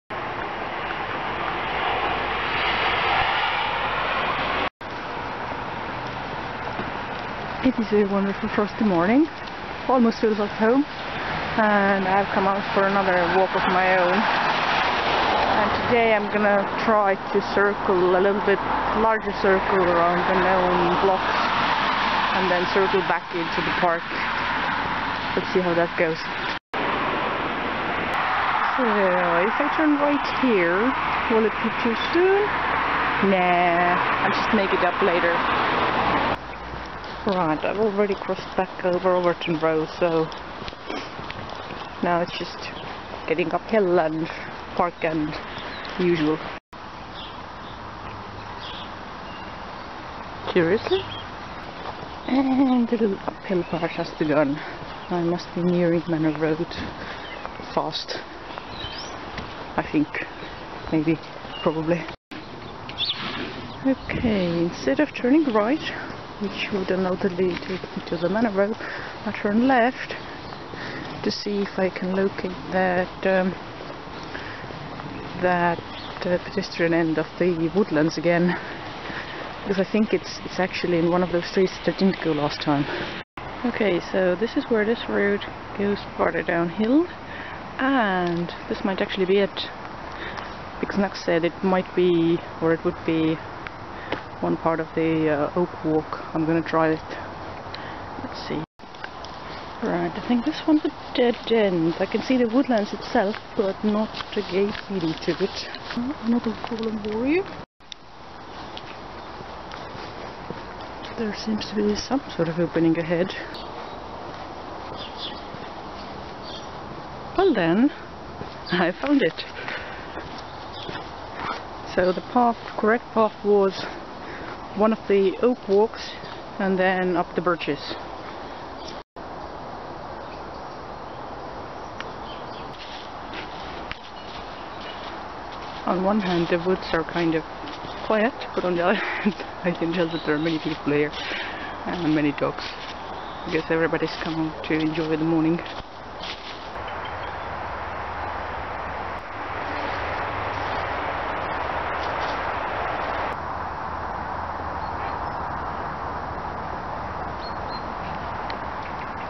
Essex expedition logs (6). A foggy Woodside morning.
On a particularly frosty morning, I explore the paths into and within Woodside park. There is also a photo album of this